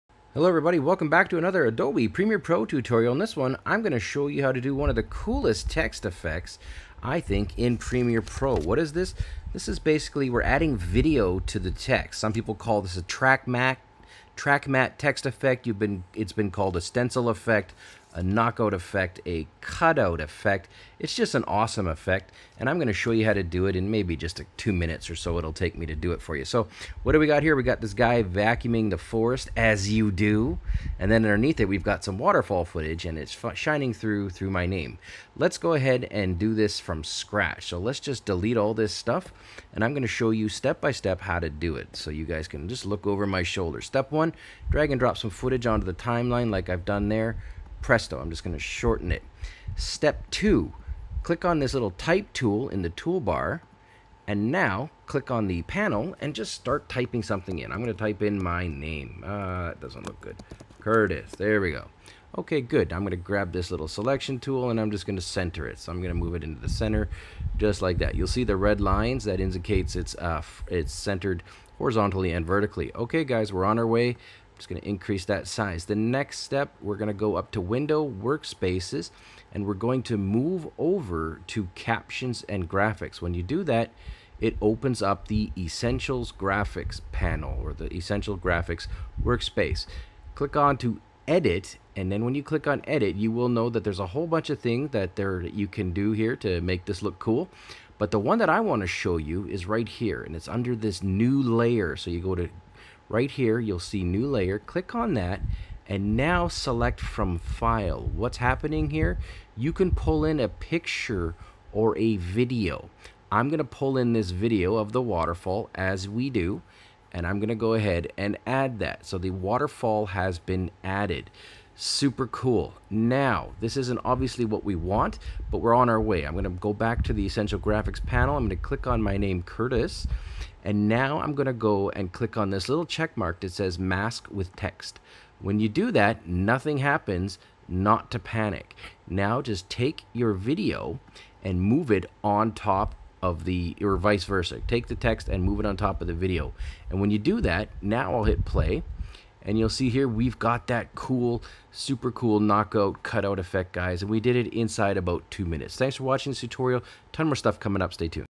How To Fill Text With Video Using Essential Graphics Panel - Premiere Pro
Filling text with video, also known as a knockout text effect, can be done in premiere pro using the Essential Graphics Panel.  The first step is to add video to your timeline.  Next, click on the type tool in your toolbar and type in the text you want to add.  Now go to the essential graphics panel and click on new layer then click on from file.  Import the video or image you want to use for the cutout effect.  Click on Mask with text, and adjust the text layer to be below the video layer.